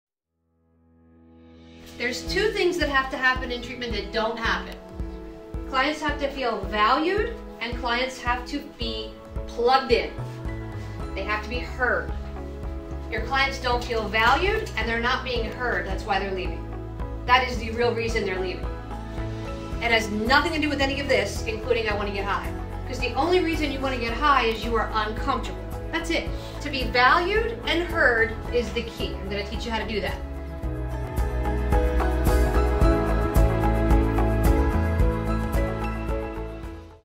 Dr. Cali Estes and The Addictions Academy - Client Value
Dr. Cali Estes speaks the importance of a client being valued and heard so that they feel comfortable in a sober living, detox or rehabilitation or addiction treatment center environment. Learn more about our training in over 30 online virtual classrooms, as well as on-site instructors!